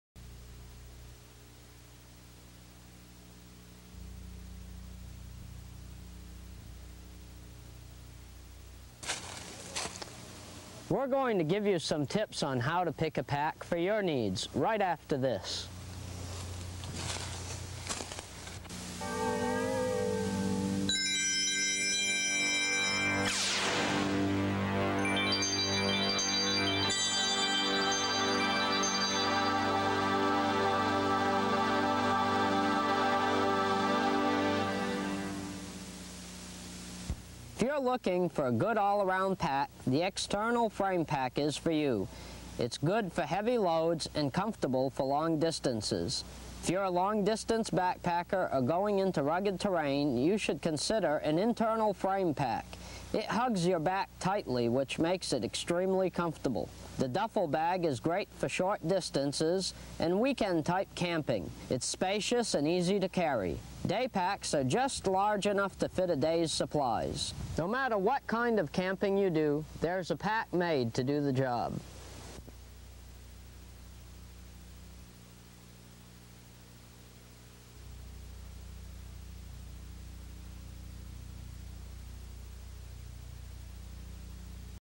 Outdoor Tips #10: Pick a Pack
In the late 1980s, Troop 160 created a series of short spots aired on local television channels, called "Outdoor Tips." It has been many years since the tips last saw the light of day... and now, thanks to modern technology, they can be seen right here on Youtube!